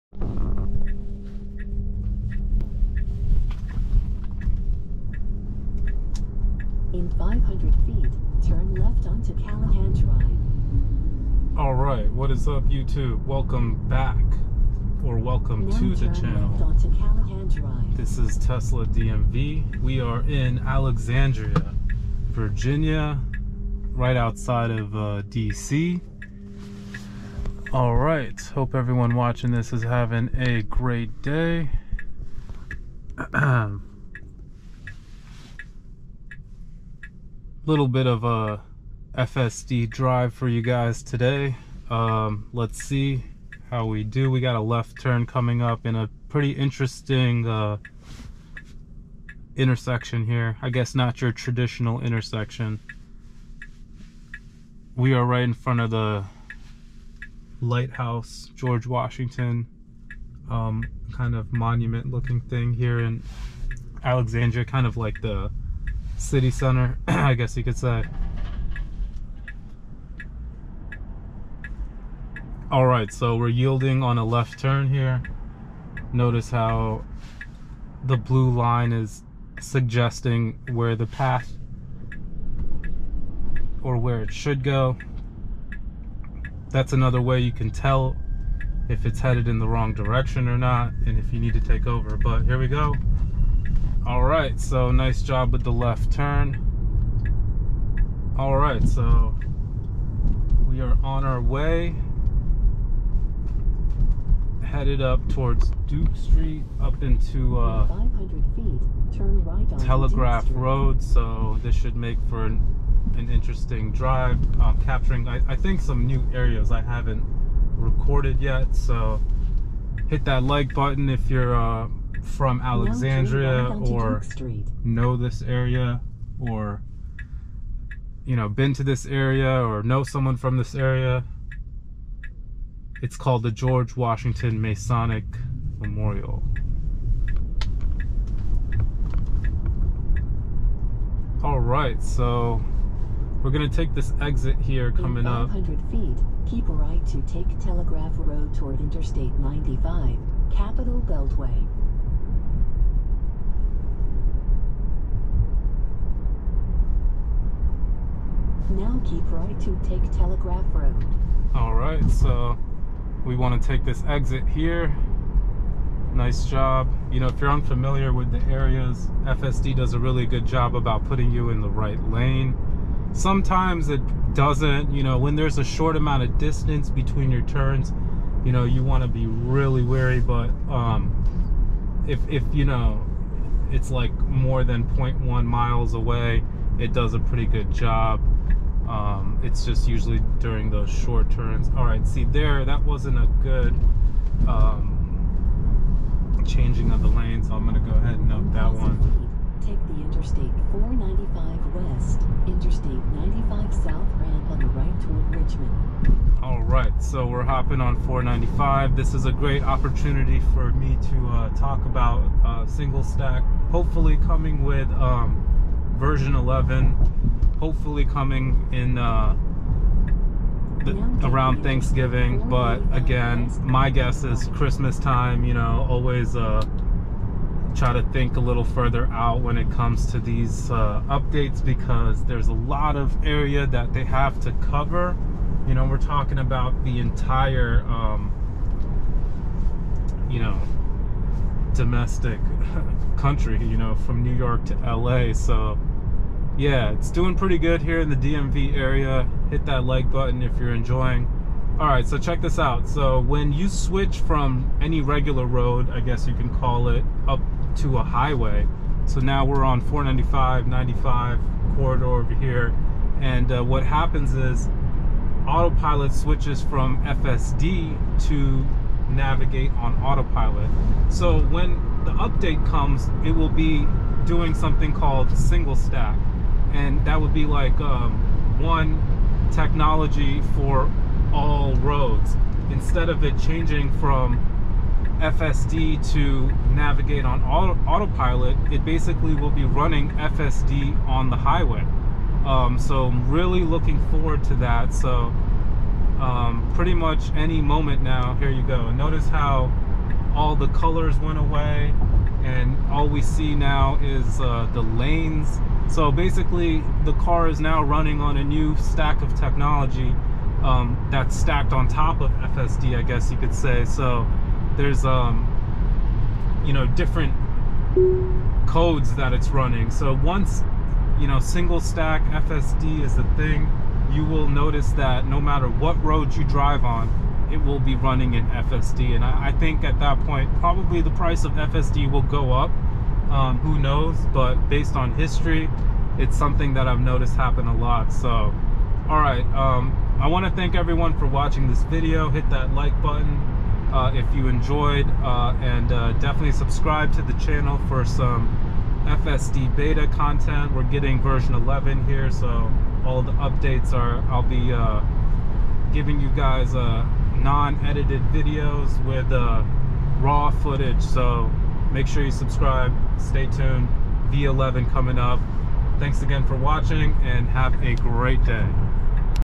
Watch Self-Driving Experiment Drive to Alexandria using a Tesla Fsdbeta single stack v11beta
what is single stack autopilot fsdbeta

Elon musk tweeted that v11beta release will get "wider" around Thanksgiving 2022